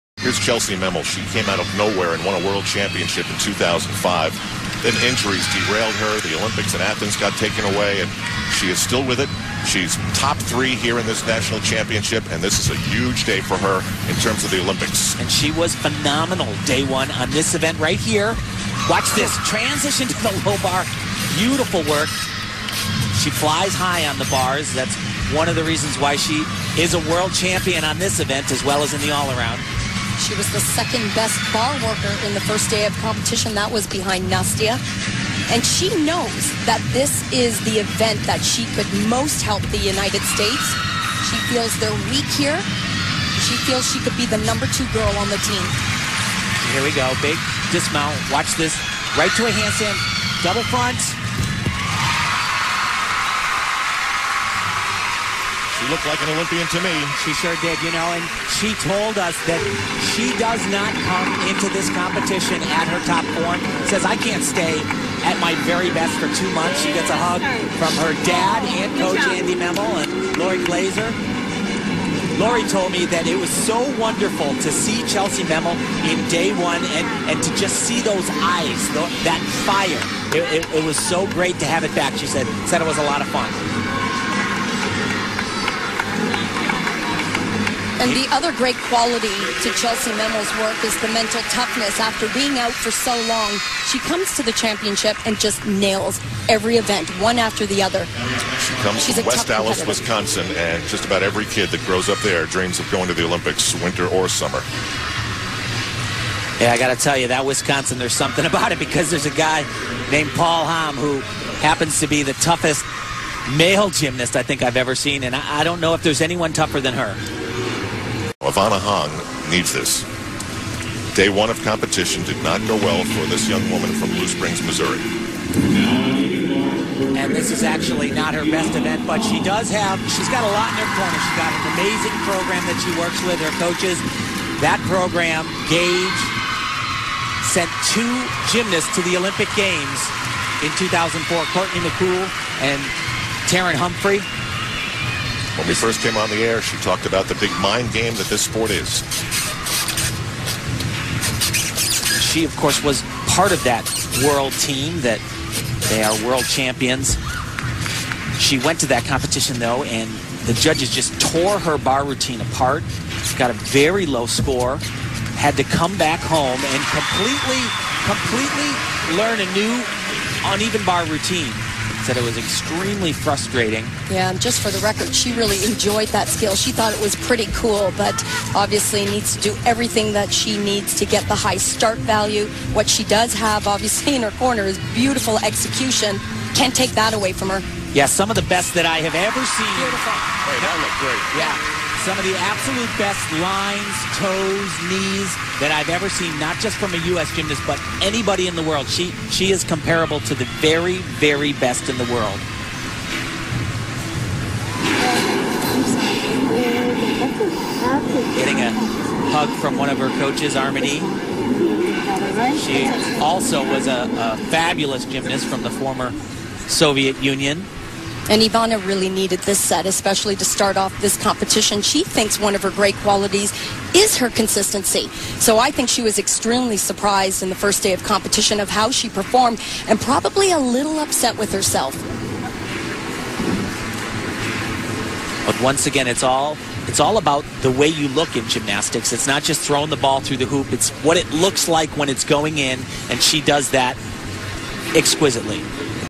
US WOMENS GYMNASTICS-UNEVEN BARS Part 1-NATIONALS Day 2-2008
NATIONAL Team Competition. Boston
Day 2. Individual Competition.
Uneven Bars. Part 1

In order of appearance:
CHELLSIE MEMMEL
IVANA HONG

Chellsie Marie Memmel (born June 23, 1988 in West Allis, Wisconsin) is an
American gymnast. She is the 2005 World All-Around Champion, making her the
third American woman, after Kim Zmeskal and Shannon Miller, to become World
Champion in the All-Around. She is also a World Champion on the uneven bars
(2003) and as part of the U.S. team (2003). She is a member of the United
States'  '08 Oly.mpic women's gymnastics team. With a total of six World medals, Memmel is one of the most decorated U.S. female gymnasts.

Ivana Hong (born December 11, 1992 in Worcester, Massachusetts) is an
American artistic gymnast. She was a member of the gold medal American team
at the 2007 World Artistic Gymnastics Championships and the all-around bronze
medalist at the 2007 Pan American Games. She was named an alternate to the
'08 U.S. Olym.pic  team.